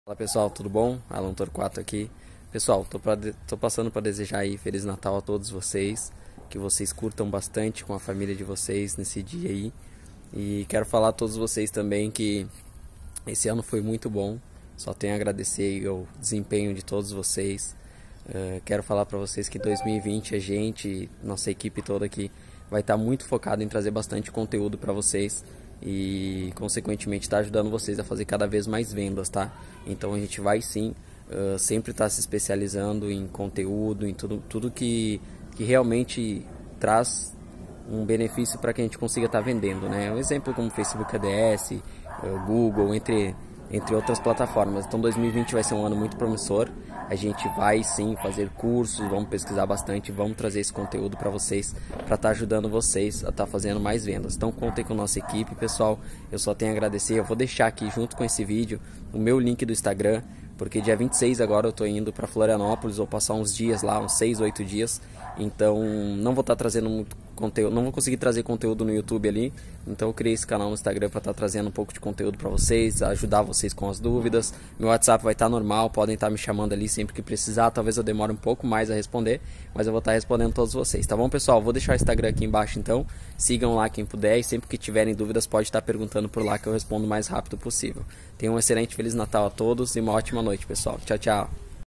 Feliz Natal a todos ! Criei um Instagram para ajudar vocês!
Pessoal fiz esse Instagram especialmente para ajudar vocês de um forma mais rápida com todas dúvidas que tenham .

E espero do fundo do coração ajudar vocês a conquistar seus objetivos em 2020.